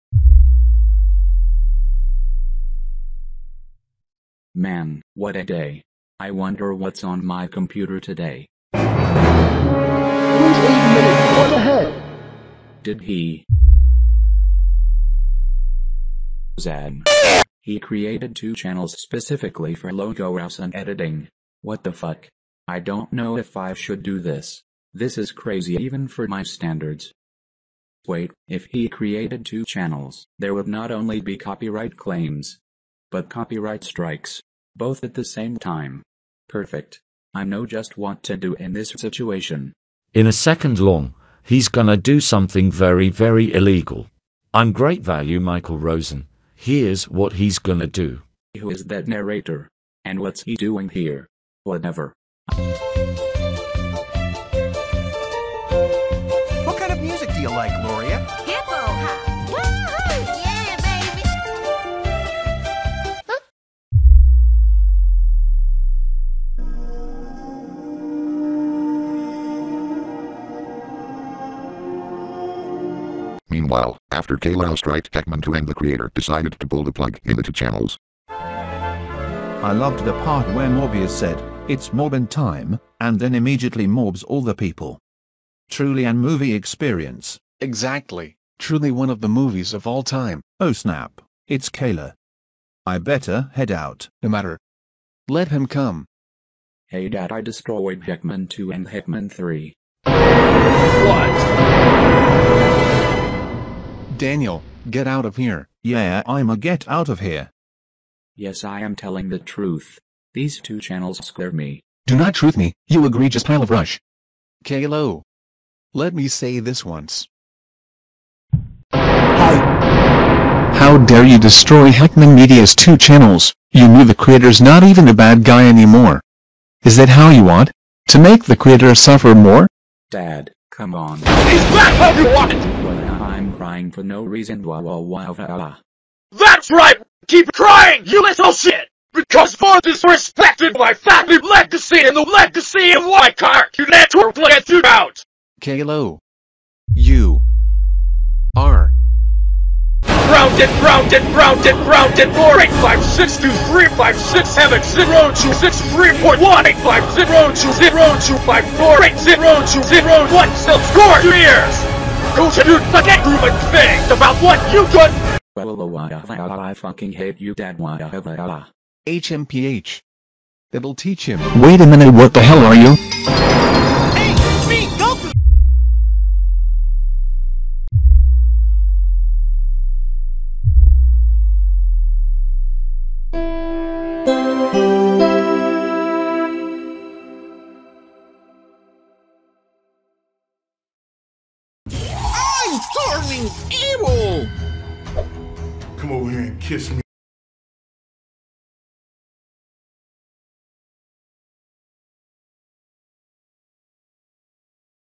Caillou destroys FT7's Alternative Channels And Gets Grounded (Based on a True Story)
based on a true yet crazy story about how I decided to move my amv content from my third channel to my main channel (except Caillou didn't Copy-strike my 2 videos on my 2nd channel, it was the greedy corporations), and it's probably one of the craziest videos I have ever done in my entire life like seriously the creator of the Heckmanimals making the first GoAnimate video i could ever do wtf is up with me (yeah I had to put the GoAnimate watermark because it could kinda feel like it was created by GoAnimate)

Never forget 🫡

Created using Wrapper Offline and Vegas Pro by HeckmanFT7 and is also not to be seen for Caillou kids and Logo Kids, nor for younger viewers as well

Note: Caillou is no longer on Cartoonito and probably because of Cartoonito's downfall due to WBD so yeah this is prob why, I should've let boris say PBS Kids idk 🤷‍♂️